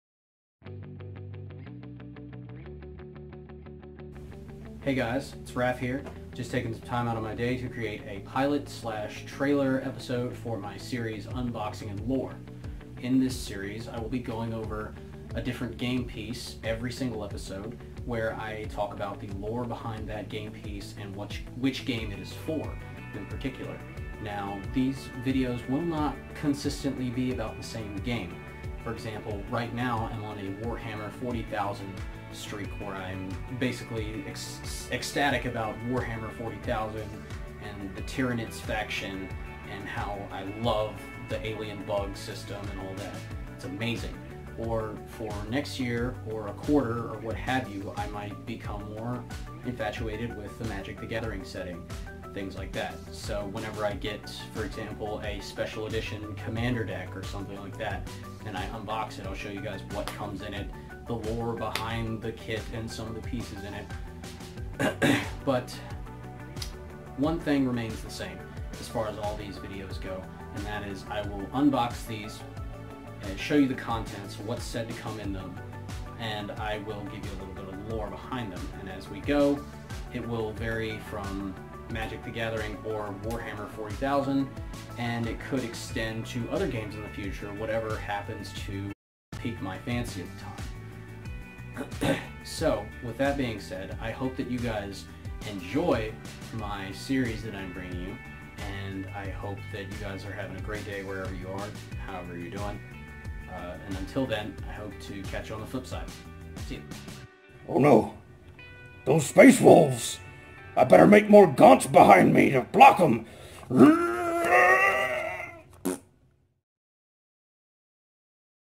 "Unboxing and Lore" trailer
Hey guys, here is my pilot/trailer video for my unboxing and lore. Better late than never! Also go check out Cino Sinan studios they are responsible for the photo of my spyro cosplay. More content to come!